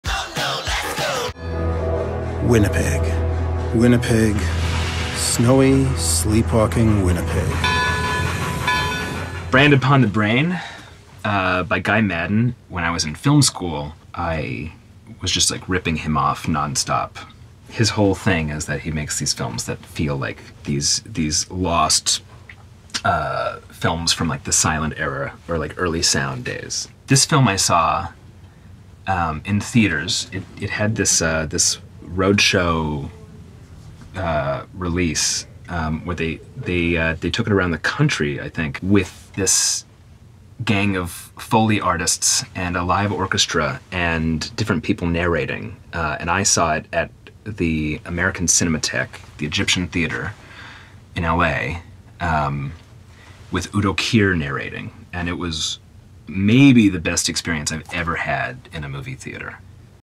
Ari Aster on Guy Maddin
Ari Aster reacts to the work of brilliant Canadian auteur Guy Maddin.